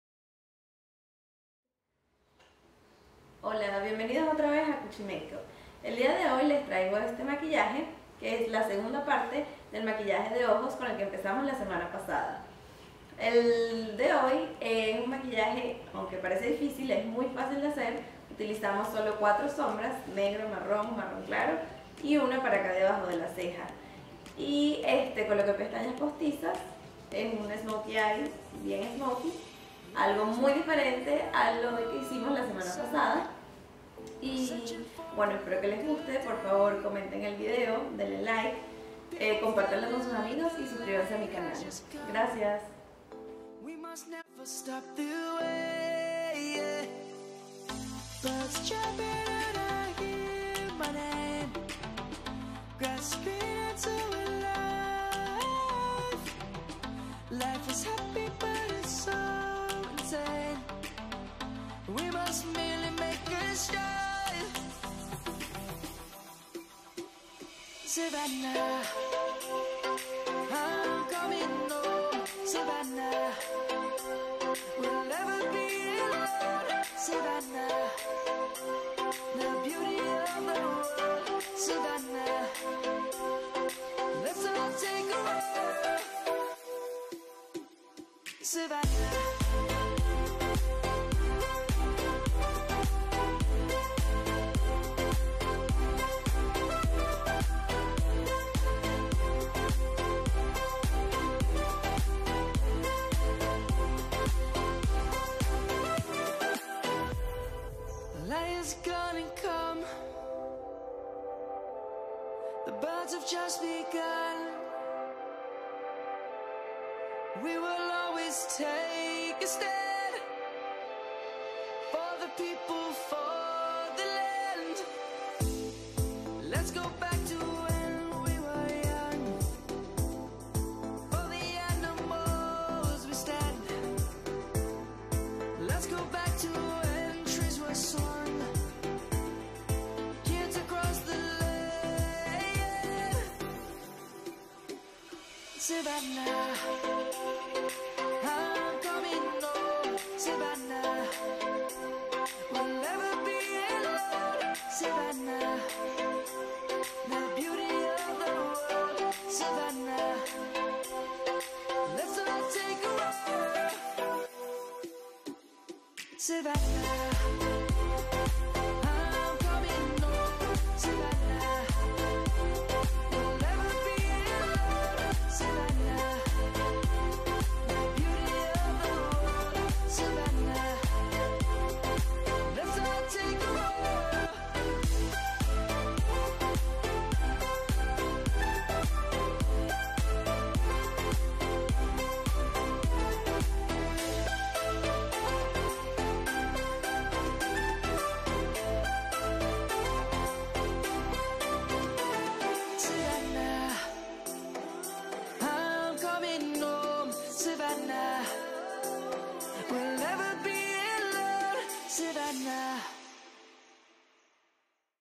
Part 2: Dramatic Smokey Eye | CUCHI MAKEUP
Hey guys! In today's video I bring you the 2nd of the 2 looks I created using the Morphe 35F palette among other products!

Products Used:
Urban Decay Eyeshadow Primer Potion (Original)
MAC Cosmetics Eye Kohl (Smolder)
Morphe 35F - FALL INTO FROST PALETTE
Eyeko London Fat Brush Mascara
KISS True Volume Lashes (Ritzy)
Duo Lash Adhesive (Dark)

Recorded with Canon EOS 70D
Edited By: Gabriela Serrano